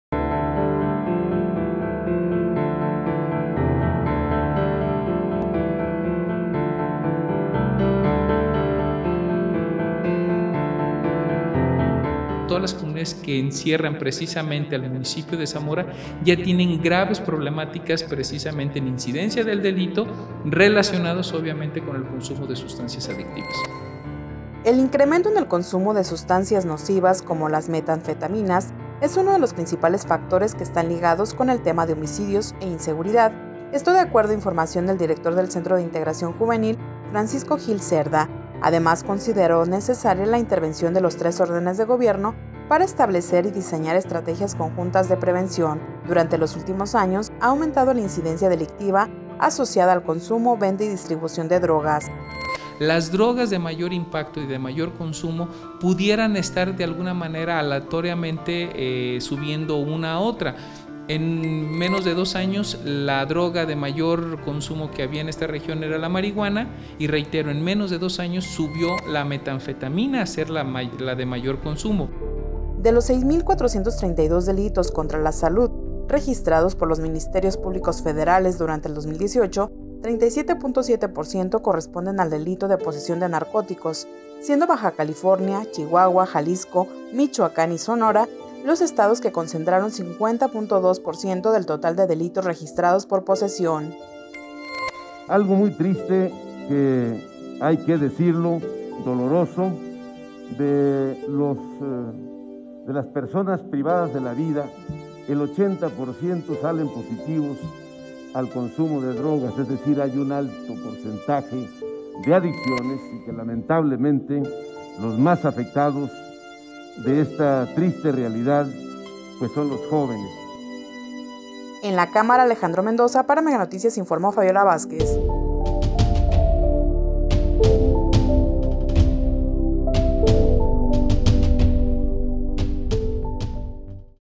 Homicidios dolosos ligados con el consumo y venta de drogas
🛑Alrededor del 80% de las muertes violentas están relacionadas con el crimen organizado. Michoacán es una de las entidades que concentra el mayor número de homicidios dolosos 😱 en el país